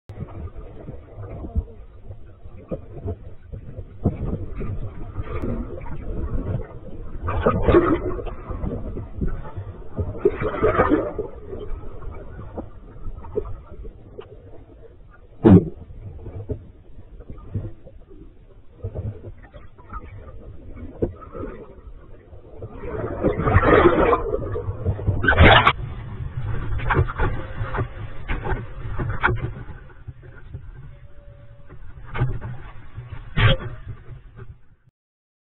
underwaterkitefoil
The Shinn Mega K foil is great equipment for kiting in conditions like we have here in Tenerife, wavy, choppy and windy.